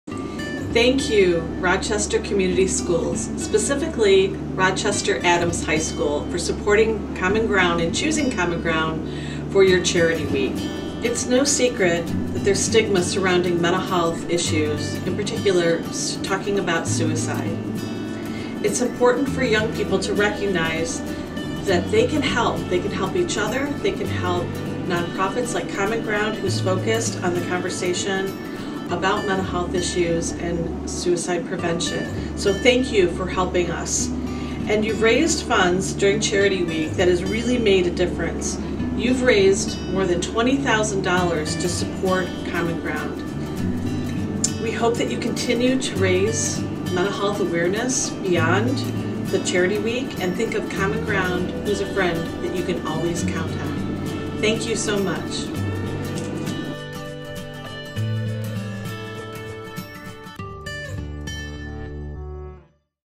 Thank You Rochester Adams High School
Common Ground would like to thank the students and staff at Rochester Adams High School for choosing our organization for their charity week!